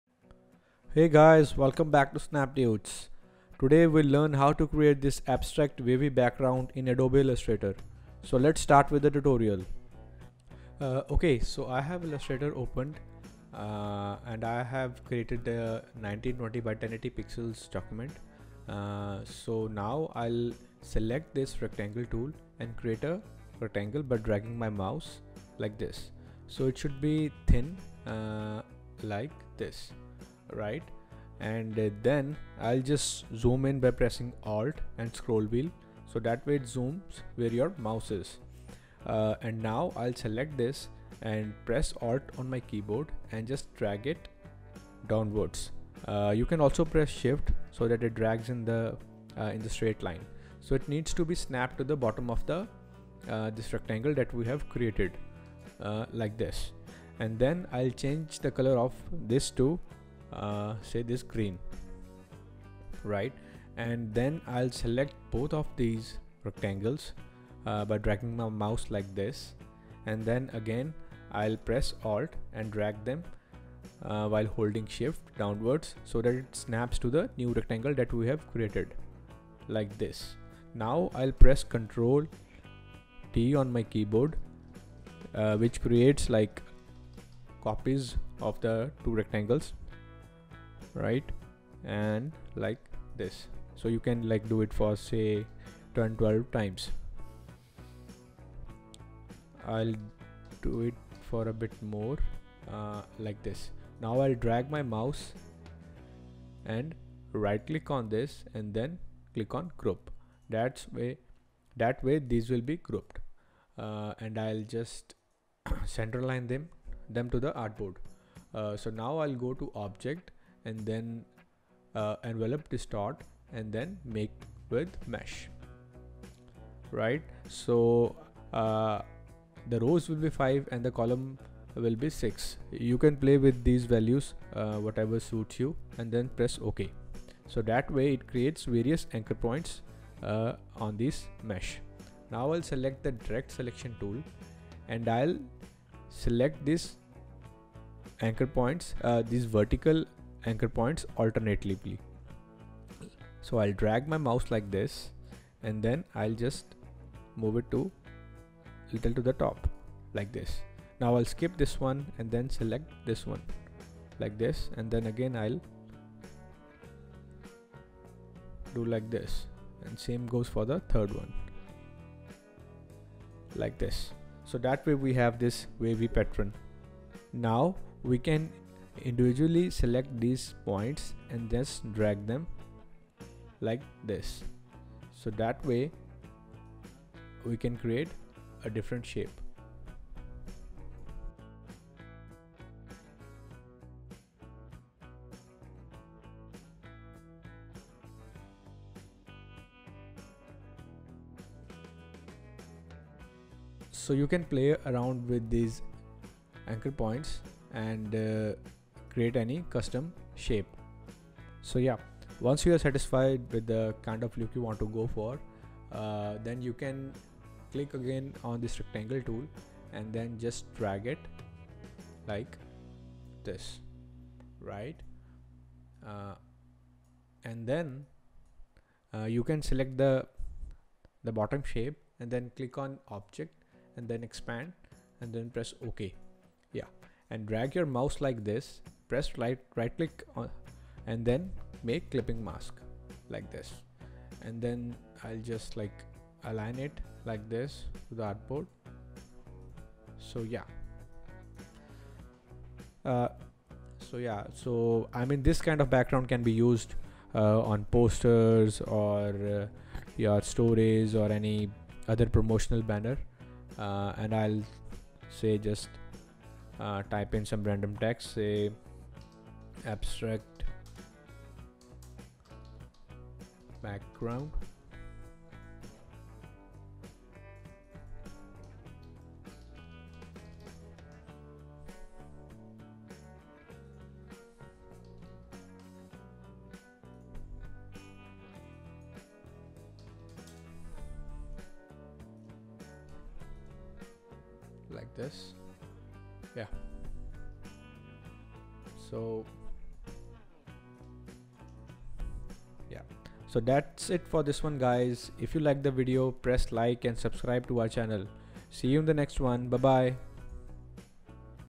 Create abstract wave background | Adobe Illustrator | Quick Tutorial
Hey guys, today I'll tell you how you can create a beautiful wavy background in Adobe Illustrator. This type of background can be used on posters, social media banners and more.

Let me know in comments if you want to see how I made this animated intro